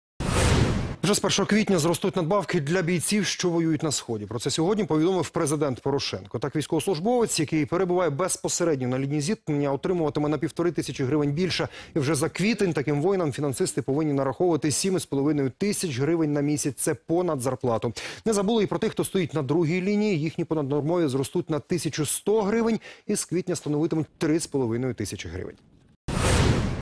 З 1 квітня зростуть надбавки для бійців, які воюють на Сході
З 1 квітня зростуть надбавки для бійців, які воюють на Сході. Так, військовослужбовець, який перебуває безпосередньо на лінії зіткнення, отримуватиме на 1,5 тис. грн більше. За квітень воїнам повинні нараховувати 7,5 тис. грн.